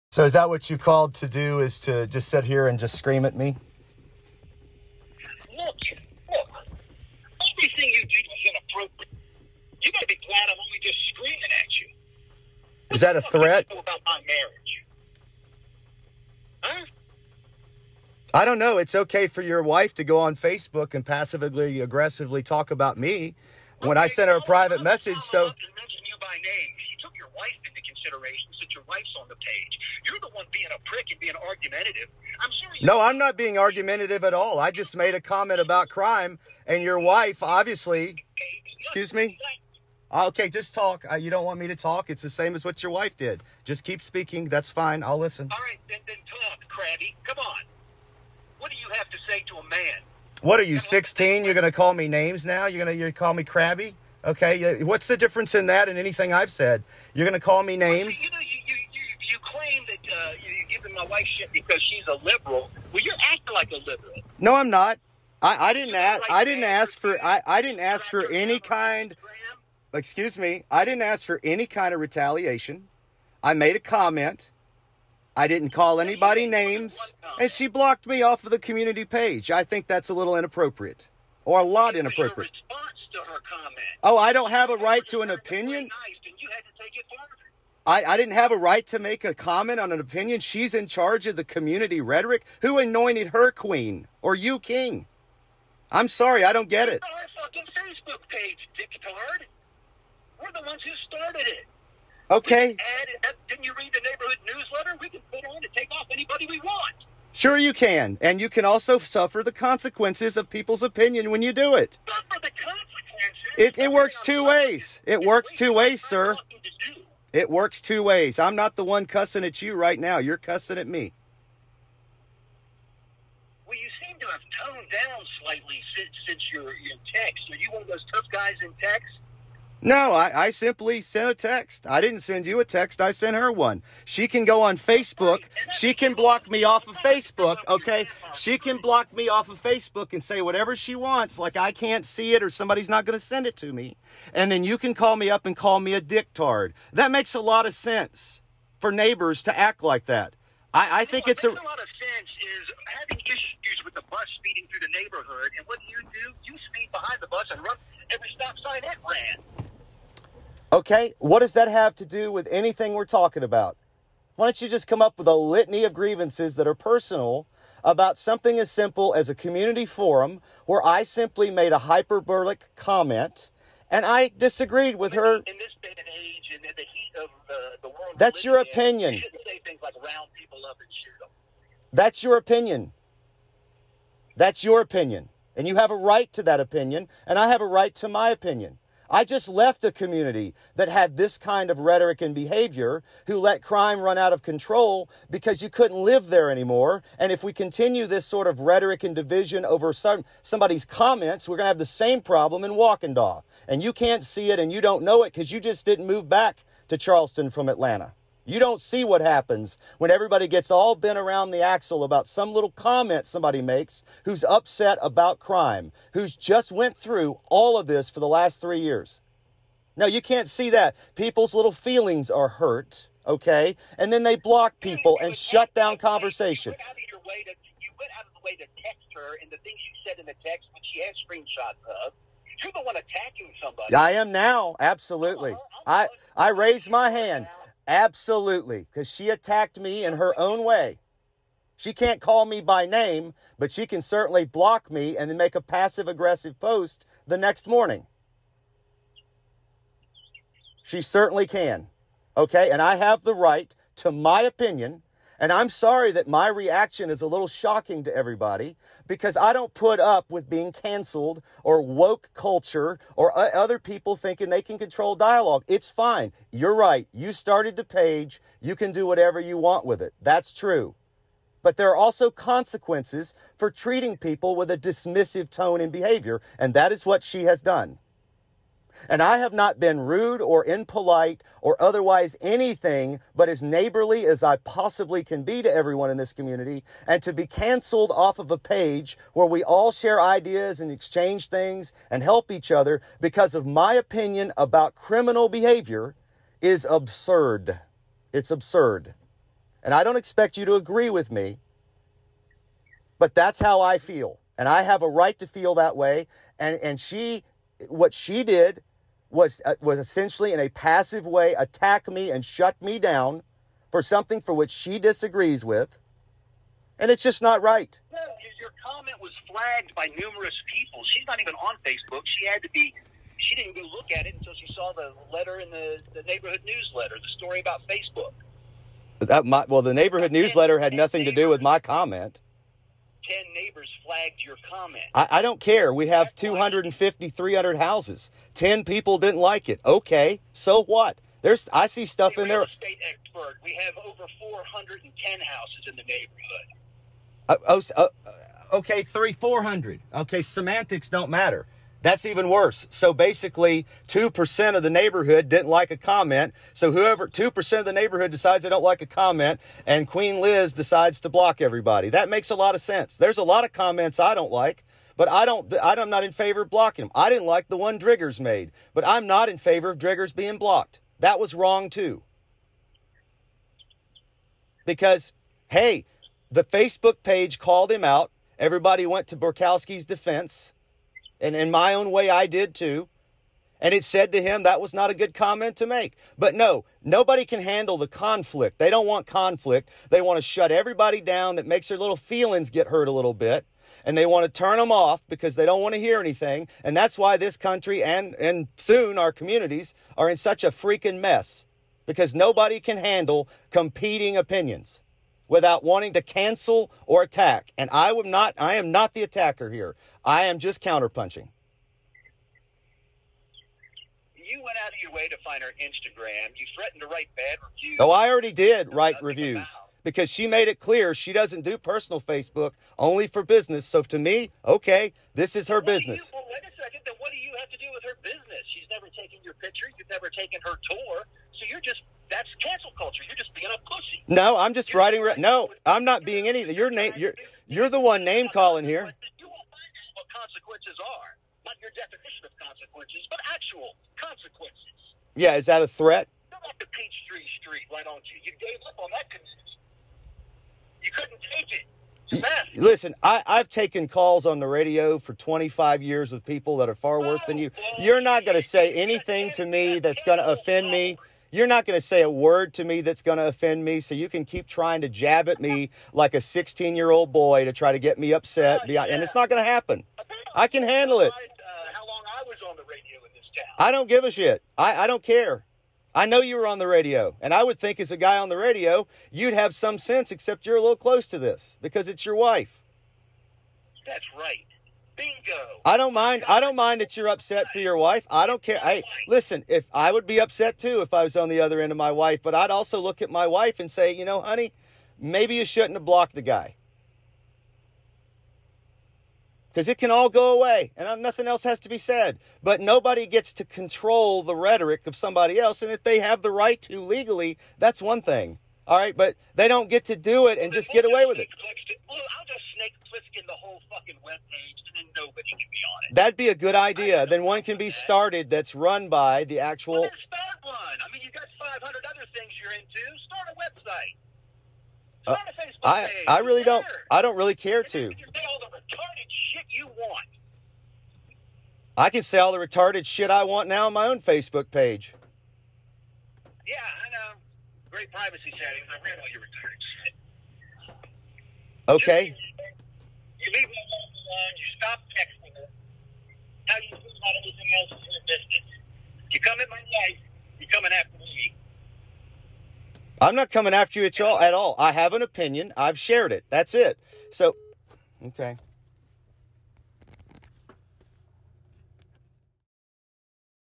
Disagree with your community Facebook page?
This is what happens when a resident like me says harsh things about criminals robbing cars in the middle of the night. You get blocked on the home owners facebook page and then when you voice your disgust, they call and bash you but they didn't expect me to be able to record it. PLEASE NOTE THAT THERE IS EXTREME VULGARITY BY THE DUREN'S IN THIS POST. PLEASE DON'T LISTEN IF YOU ARE OFFENDED BY THE LANGUAGE.